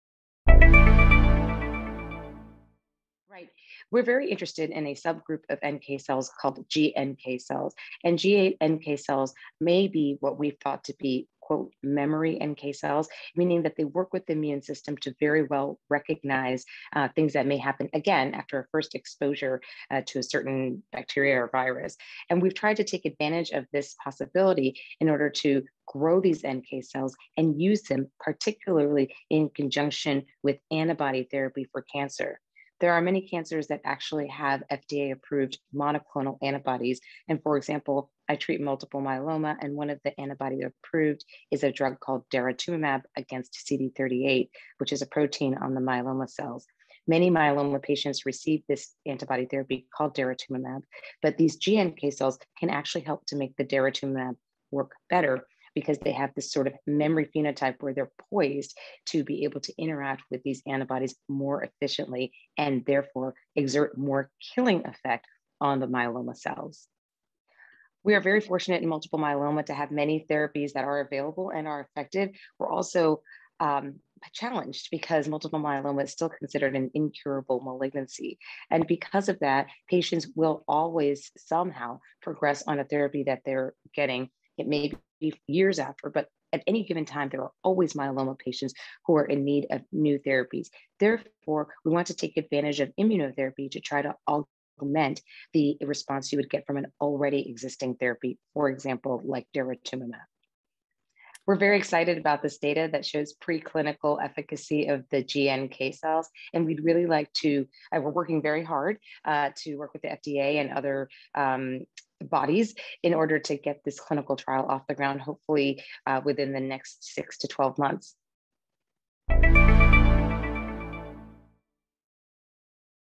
Natural Killer Cells as Add-on Therapy for Multiple Myeloma is Showing Promise in the Lab
Nina Shah, MD, Hematologist from the University of California, San Francisco, discusses preclinical data demonstrating that a subset of  human natural killer (NK) cells that lack expression of FcεRIγ (g-NK cells) enhance the efficacy of monoclonal antibody treatment in multiple myeloma patients.

Multiple myeloma is a rare blood cancer associated with uncontrolled growth of myeloma cells. While the disease is treatable, relapses are common and some patients are refractory to first line treatment.
As Dr. Shah explains, g-NK cells are potentially what was previously known as “memory” NK cells as they work with the immune system to recognize viruses and bacteria. In a preclinical study led by Dr. Shah, the ability of g-NK cells to improve the efficacy of monoclonal antibody treatments (e.g., daratumumab or elotuzumab) in multiple myeloma was examined. Results from in vitro assays showed g-NK cells had six-fold higher anti-myeloma cell-killing activity compared with conventional NK cells when combined with daratumumab or elotuzumab.

Additionally, in a mouse model of multiple myeloma that investigated tumor regression, the combination of g-NK cells and daratumumab led to a 99.9% reduction in tumor burden compared with the combination of conventional NK cells and daratumumab. At Day 57 post-tumor inoculation, all 7 mice treated with the g-NK cells in combination with daratumumab were alive, and the myeloma tumor burden was eliminated in 5 of 7 mice.

In contrast, on Day 57, no mice treated with conventional NK cells and daratumumab were alive. Further, as the cancer was allowed to grow for two weeks post-inoculation until therapy was started, this preclinical study resulted in a cancer regression model as opposed to a cancer suppression model, as is often seen in other preclinical NK cell studies.

According to Dr. Shah, Indapta Therapeutics hopes to have a clinical trial testing g-NK cells as a multiple myeloma treatment in the next 6-12 months.